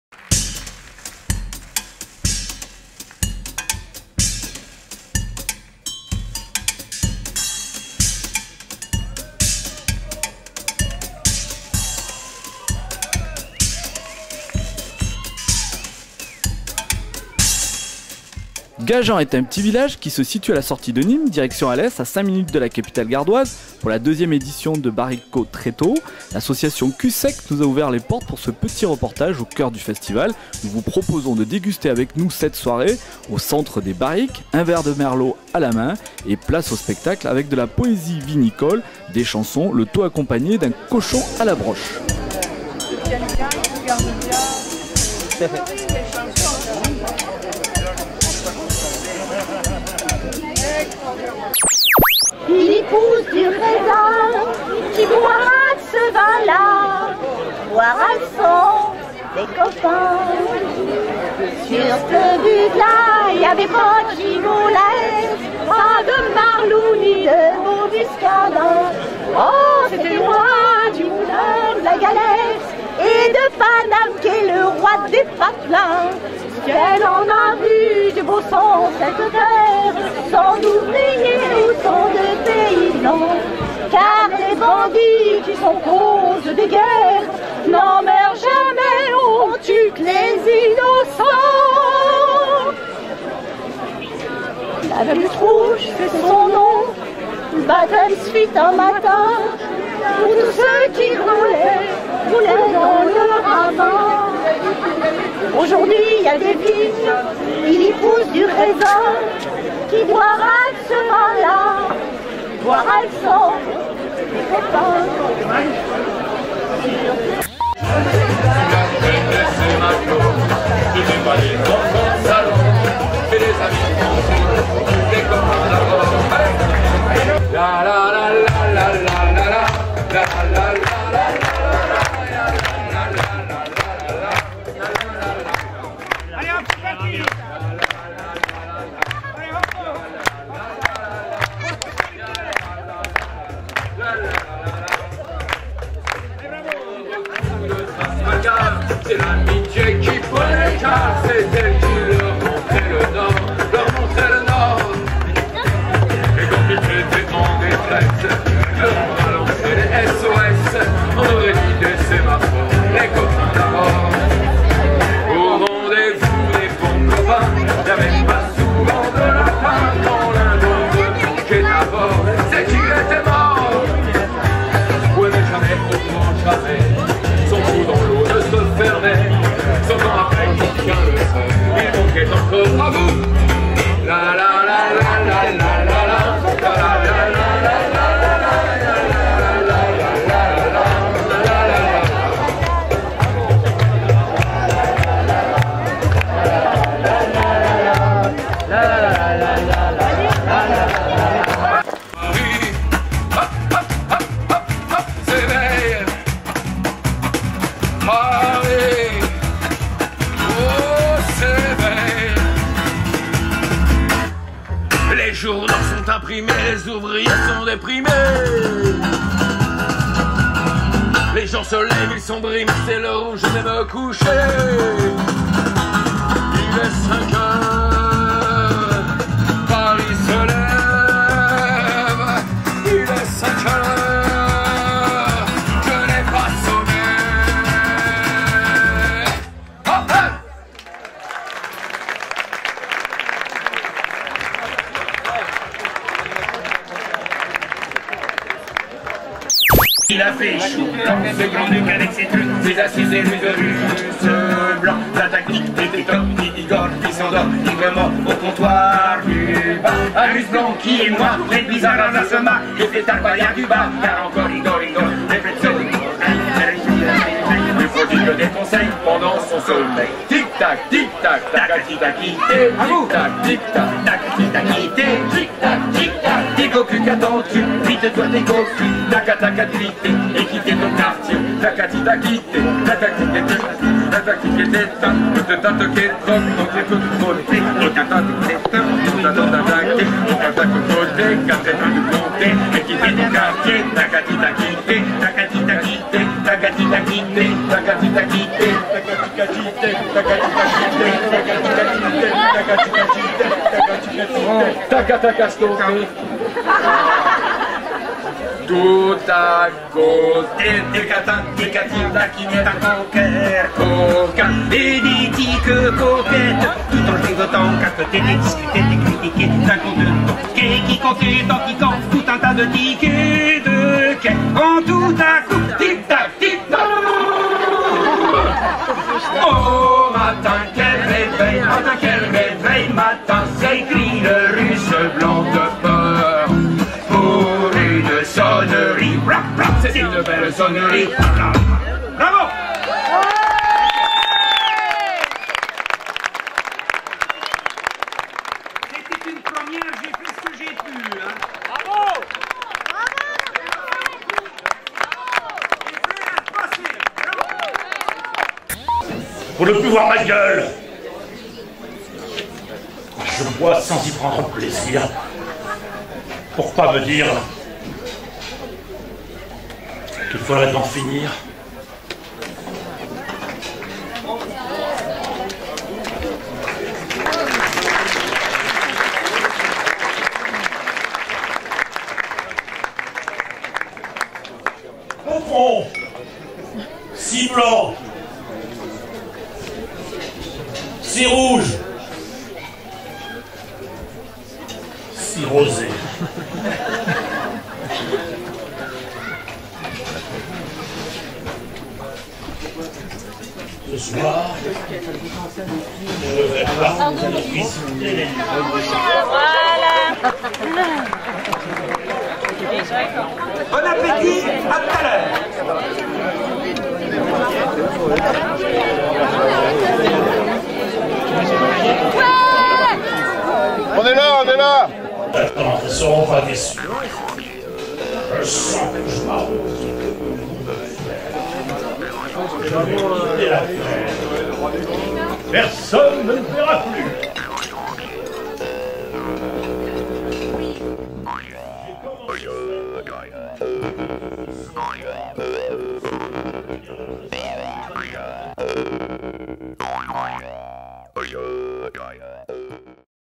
Barikôtreto Vin & spectacle (Gajan 2011)
Pour cette 2ème édition, Les" Frères à repasser" font leur répertoire musical face à un public averti !!! Conteurs & musiciens partagent la fête du cochon.